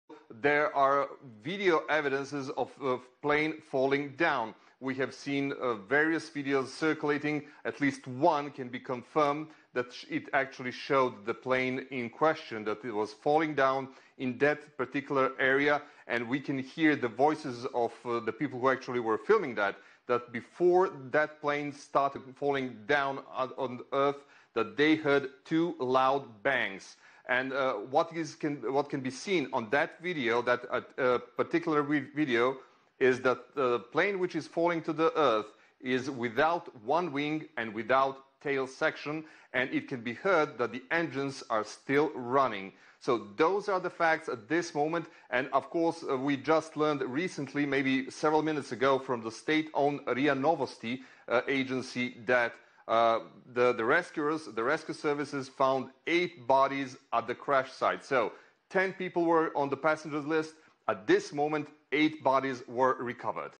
BREAKING NEWS: Witnesses of Prigozhin plane crash who filmed incident heard two loud bangs
A plane believed to be carrying Wagner Group leader Yevgeny Prigozhin has crashed on a flight from Moscow to St Petersburg with 10 people on board. Reporting from Moscow Aljosa Milenkovic reveals eye witnesses heard ‘two loud bangs’ before the plane began to fall front he sky and that so far two bodies are unaccounted for.